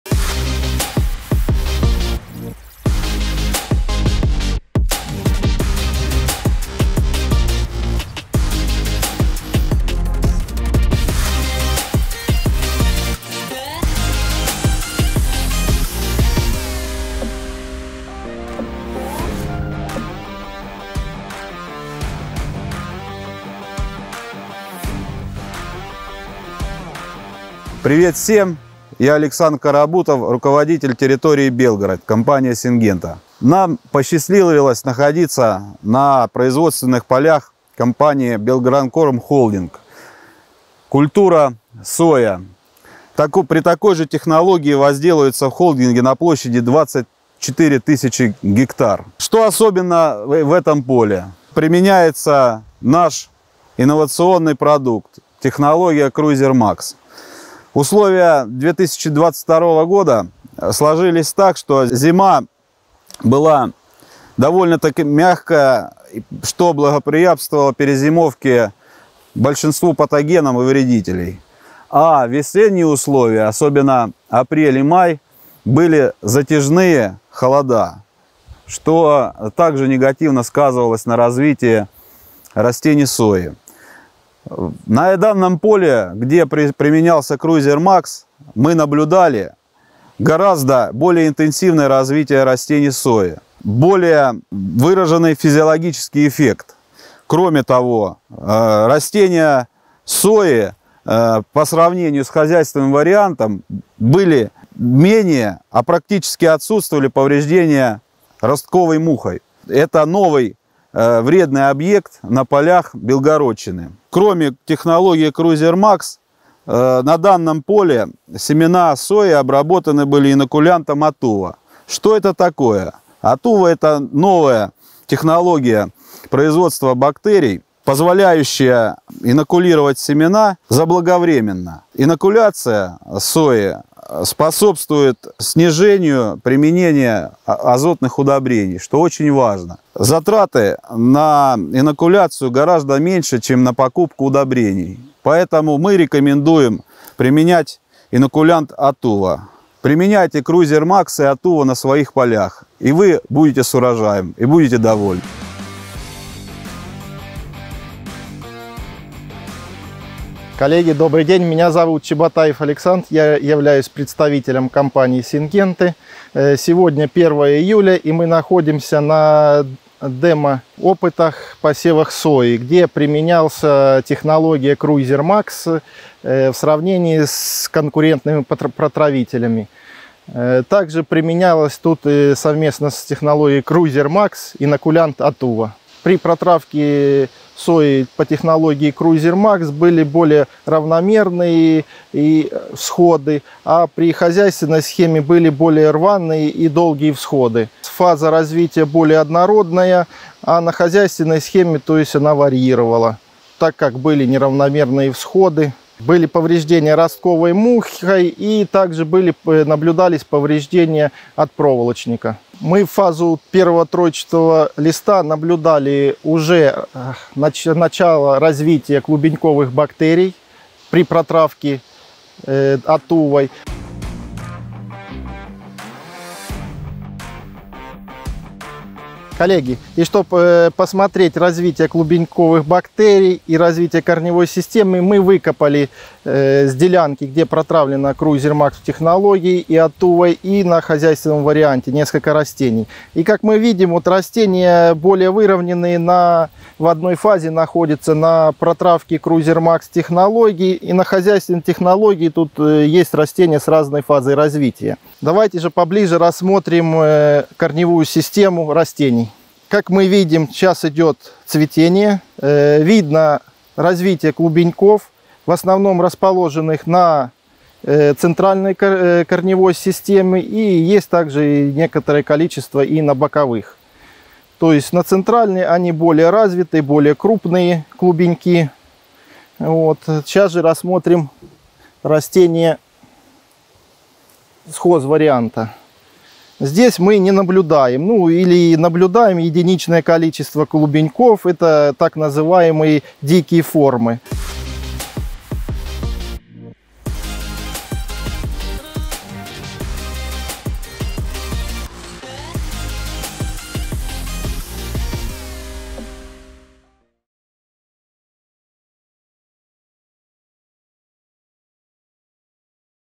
СОЯ PRO: ОТ ЗАЩИТЫ ВСХОДОВ ДО КЛУБЕНЬКОВ. ПОЧЕМУ ВЫГОДНО ПРИМЕНЯТЬ ИНОКУЛЯНТЫ
В агропромышленном холдинге «БЭЗРК-Белгранкорм» в текущем сезоне приняли решение обработать свои семена сои продуктом КРУЙЗЕР® Макс-технология и инокулянтом АТУВА®. Эксперты компании «Сингента» в этом ролике наглядно демонстрируют количество клубеньков, образовавшихся на растении при обработке инокулянтом АТУВА® в сравнении с хозяйственным вариантом. Несмотря на холодные условия, обработка продуктами «Сингенты» позволила получить равномерные и дружные всходы, находящиеся в более однородной фазе развития.

В серии выпусков СОЯ PRO представители ведущих соевых предприятий Центральной России будут делиться опытом применения на своих полях продуктов для защиты семян компании «Сингента».

- Больше информации о КРУЙЗЕР® Макс-технологии